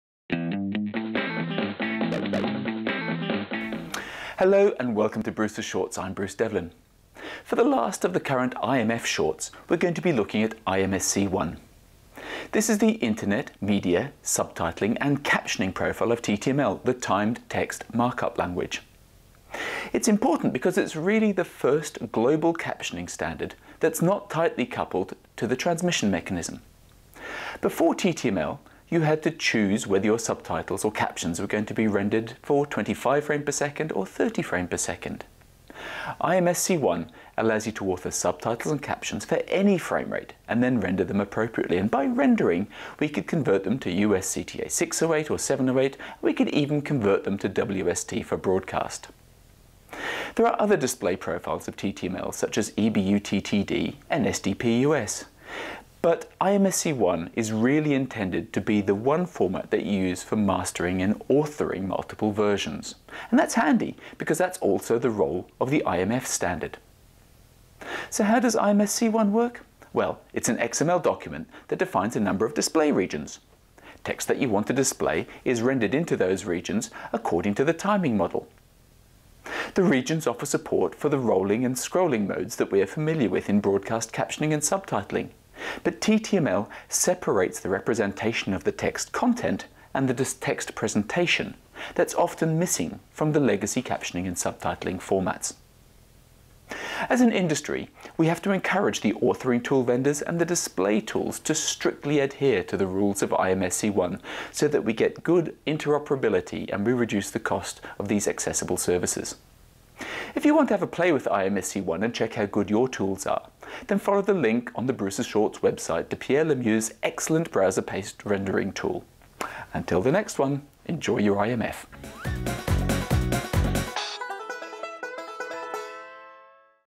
bs405 IMF Captions & Subtitles - IMSC1
IMF needs a generic captioning and subtitling format for the planet. Why choose IMSC1